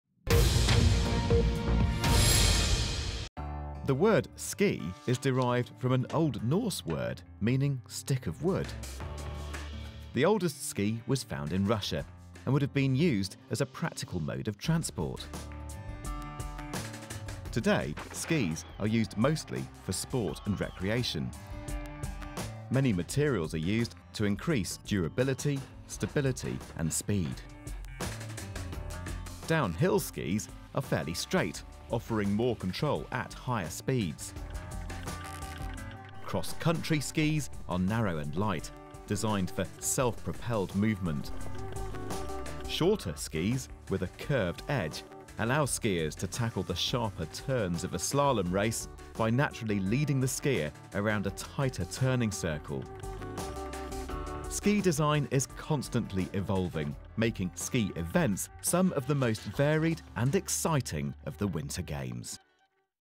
Science of Sochi: Ski development - BBC News
The ski has a long history but hasn't always been associated with competition, as we know it. Modern ski events are as varied as they are exciting, often requiring bespoke skis that are designed to fit the bill.